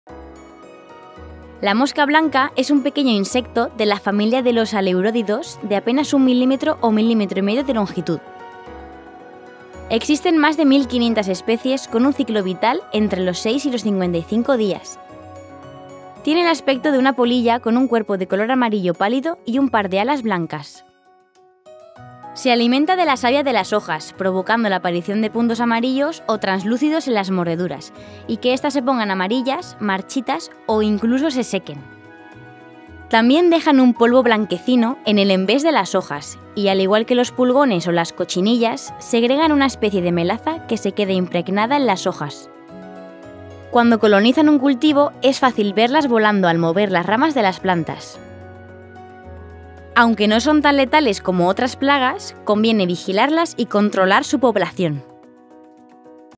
MOSCA BLANCA PLAGA EN PLANTAS
🚨🚨 PRECAUCIÓN 🚨🚨 🌱

Existen diferentes variedades de mosca blanca, pero la más habitual en los cultivos de marihuana es la Trialeurodes Vaporiorum. Suele invadir los cultivos de interior, o los cultivos de exterior en climas cálidos.

Como mínimo, en exterior suelen darse cuatro generaciones al año. En interior, donde el ambiente está más controlado, pueden llegar a tener hasta 12 generaciones al año, es decir una generación al mes.

Lo más efectivo para controlar esta plaga es hacerlo a través de la lucha biológica.